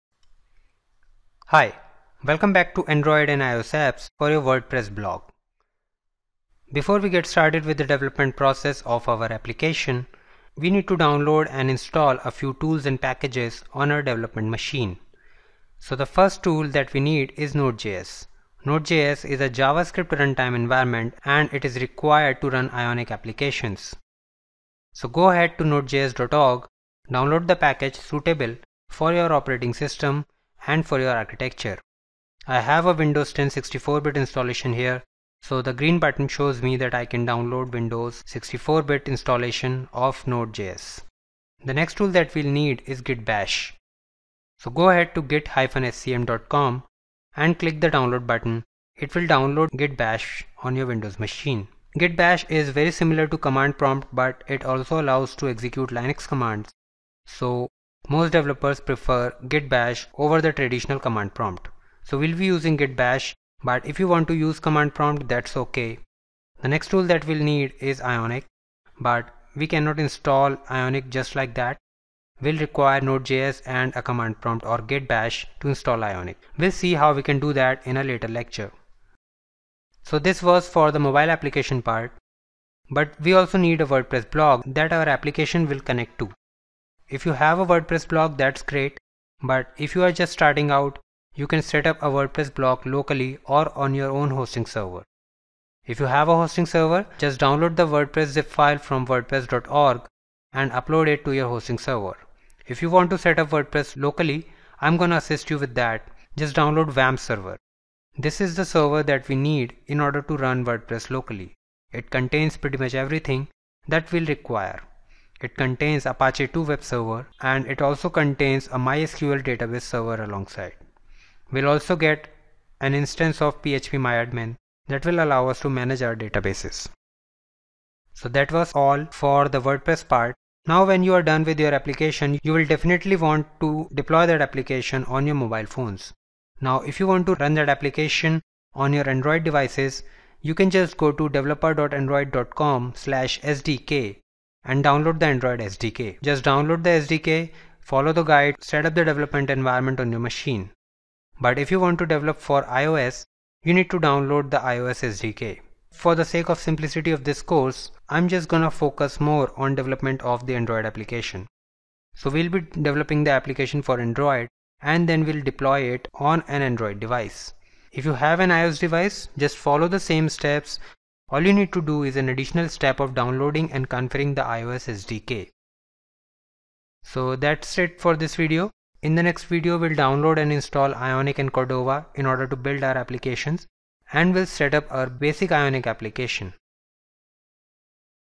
Android and iOS Apps for Your Wordpress Blog : Tools and Packages
Android and iOS Apps for Your Wordpress Blog
Build a mobile app that interacts with your Wordpress Blog. Use HTML, CSS and JS to build you app for Android and iOS.